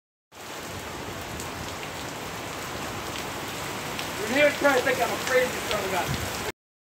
Redneck way of taking a shower!!
My cousin taking a shower in the rain fall.